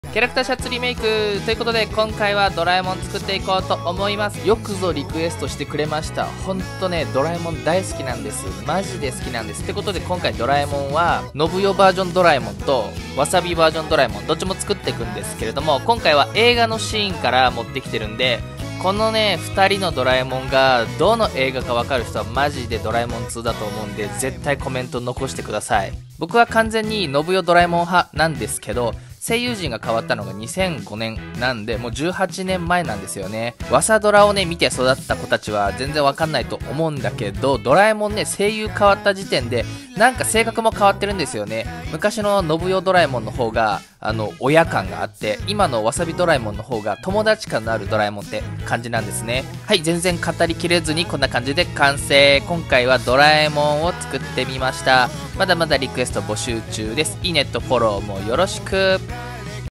キャラクターシャツリメイク！今回は大山のぶ代ドラえもんと水田わさびドラえもんを作ってみた！
GUのシャツをリメイクしていきます！今回はみんな大好きドラえもんをのぶ代ドラえもんverとわさびドラえもんverを作ってみた！
リクエストくれた方々ありがとうございます！！
みんなの好きなキャラクターをスキャンカットで切り出してリメイクしていきます！